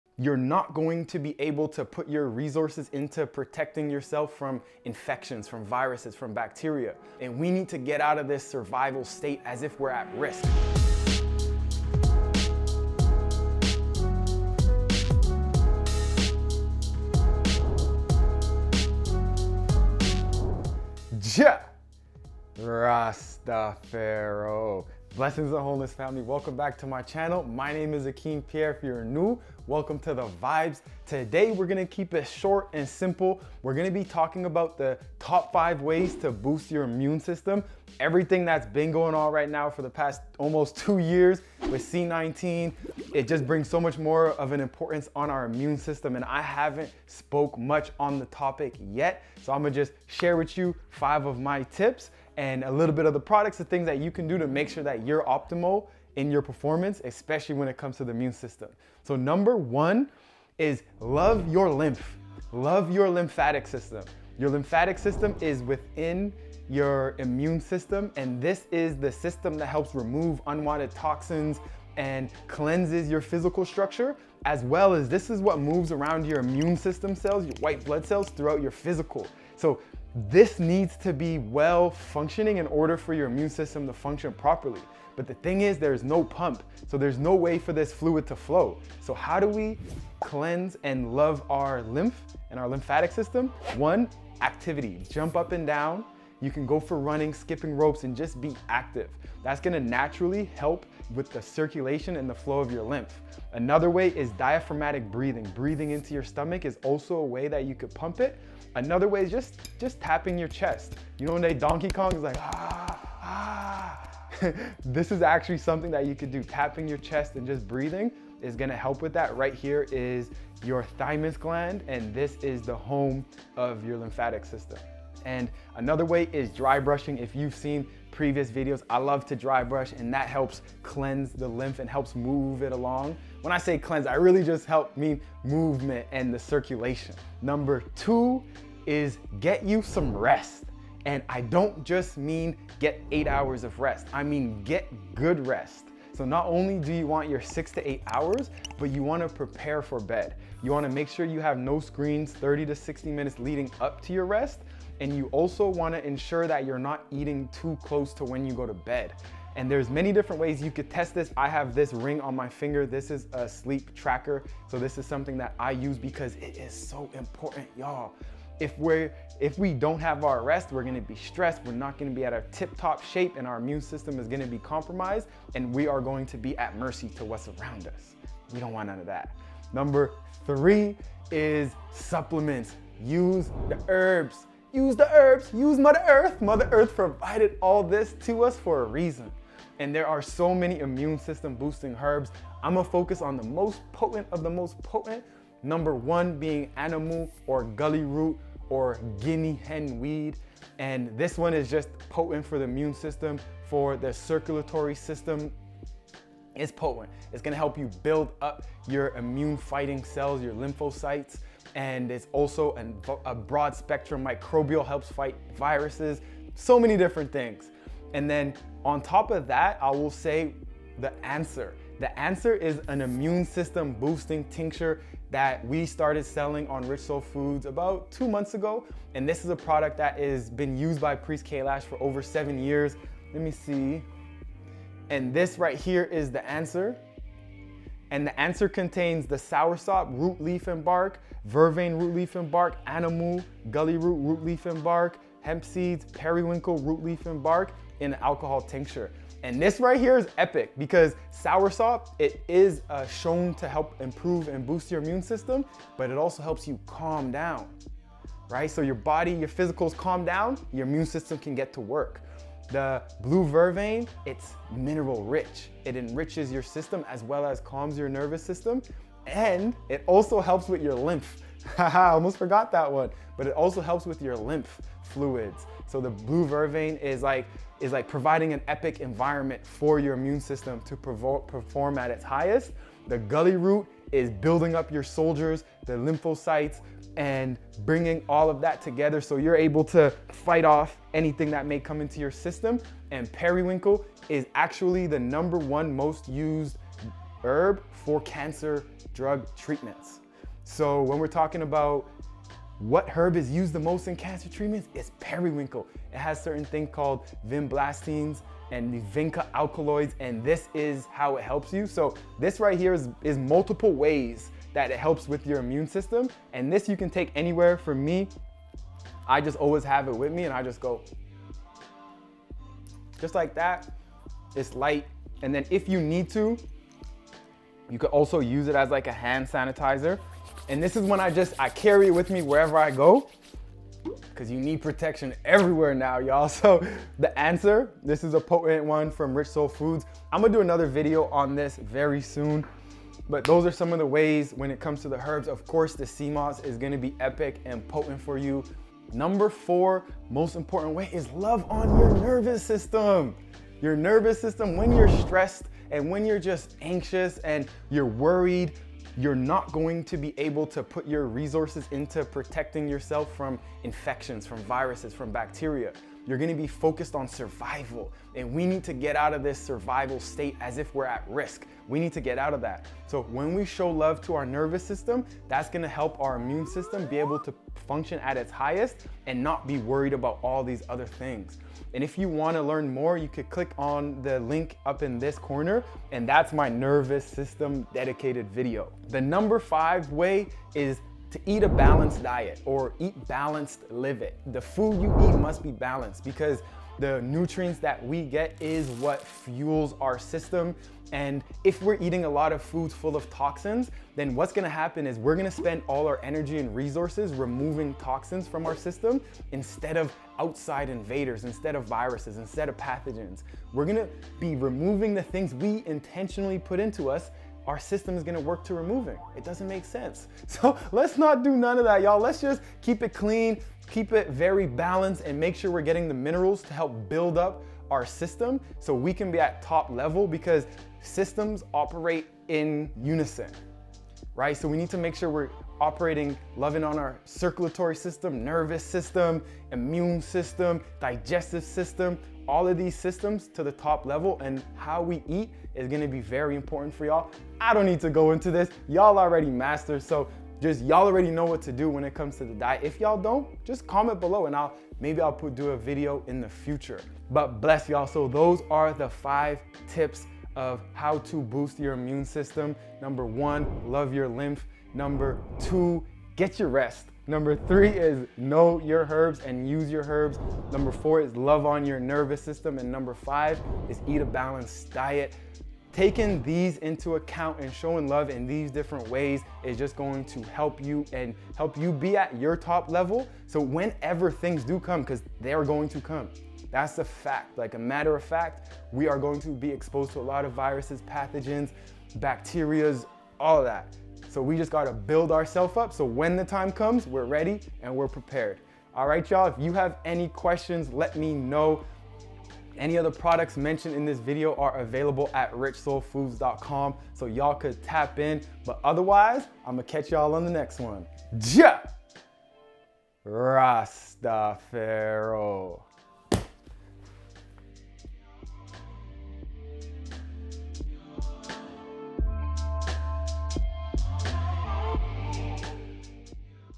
5 Ways To Boost Your Immune System The High Grade Healing Way
Blessed love y’all! Here are share some quick tips on how to level up your Immune system! Let me know If you learned Anything and also how you like these straightforward how-to videos!

☀️ Social
Akeem:

☀️ Getting in touch w/ me

Before you get in touch  ⇢ Please know that I am only able to get back to business opportunities that match my schedule and interests.

1:02 #1 Love Your Lymph
2:21 #2 Get You Some Rest
3:05 #3 Use the Herbs
5:52 #4 Love Your Nervous System
6:33 #5 Eat a Balanced Diet
7:45 Recap